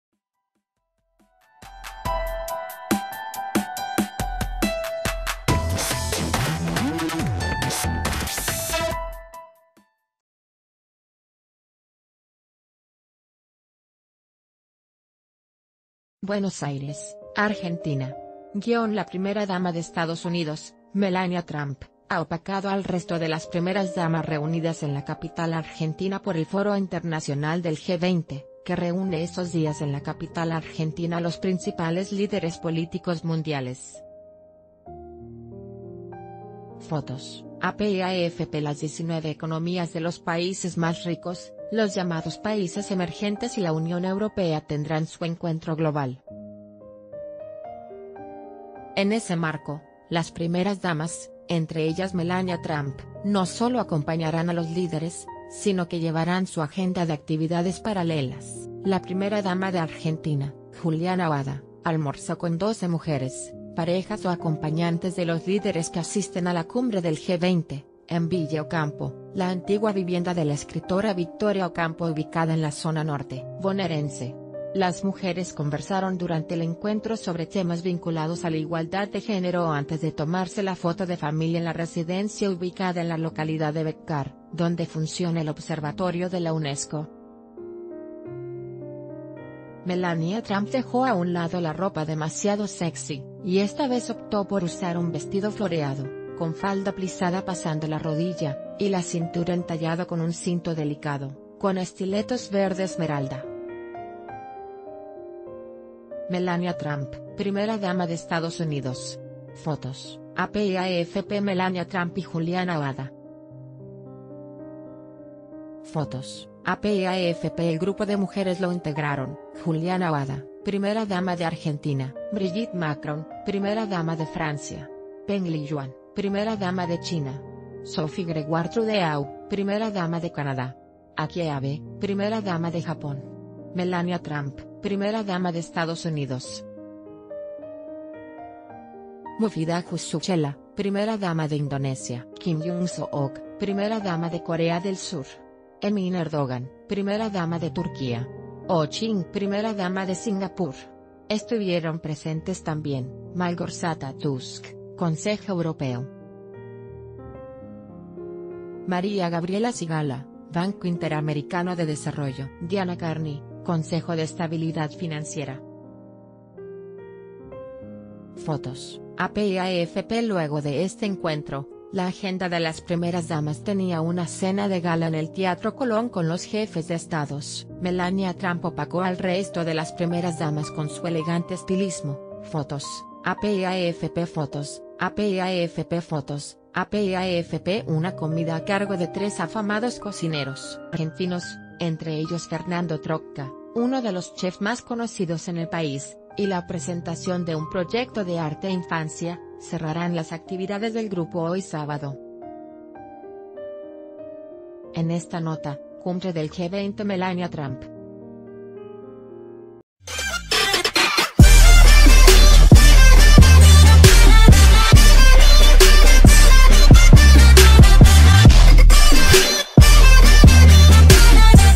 Melania Trump opaca a las Primeras Damas en el G20
Buenos Aires, Argentina.- La Primera Dama de Estados Unidos, Melania Trump, ha opacado al resto de las Primeras Damas reunidas en la capital argentina por el foro internacional del G20, que reúne estos días en la capital argentina a los principales líderes políticos mundiales. Fotos: AP y AFP Las 19...